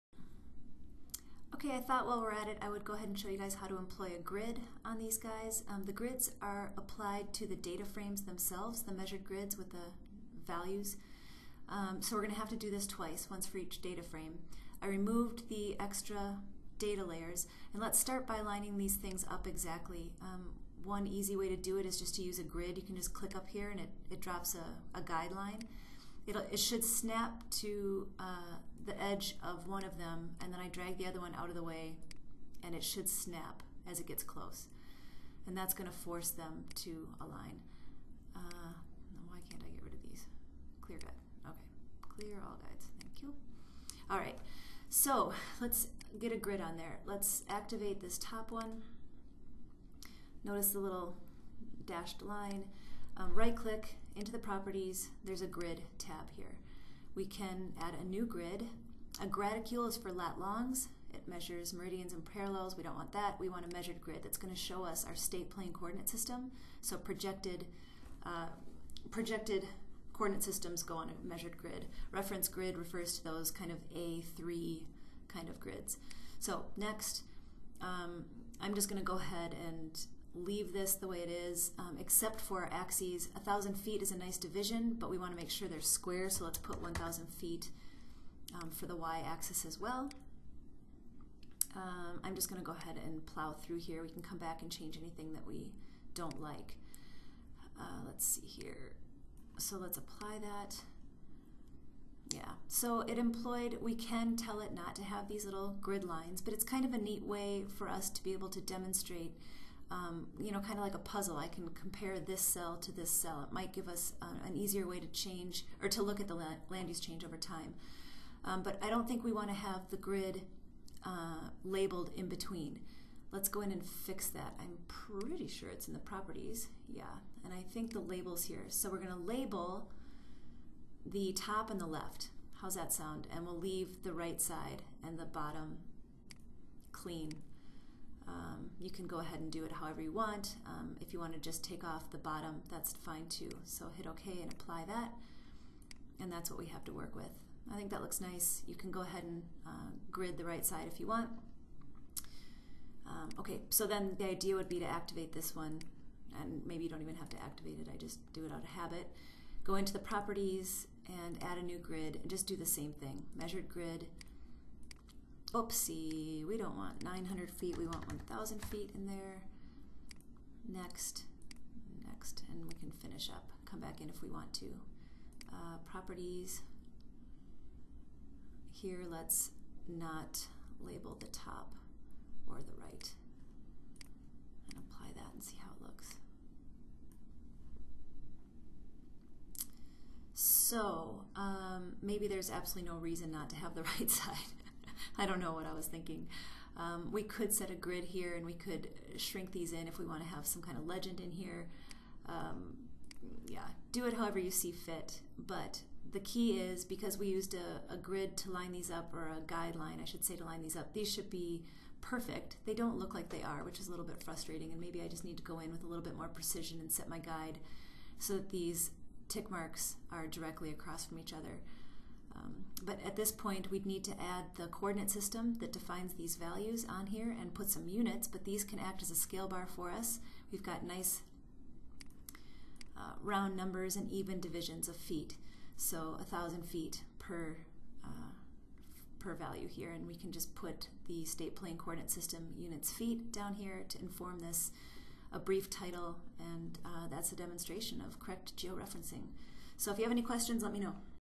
How to Add a Measured Grid to a data frame.
Two obvious corrections: 1. set up one data frame exactly the way you want it first, add the grid THEN copy it to save time and work, 2. use the align features to align the grids by selecting both data frames, right clicking, and aligning that way.  Remember to include the coordinate system of the grid values (and units) or it has no meaning.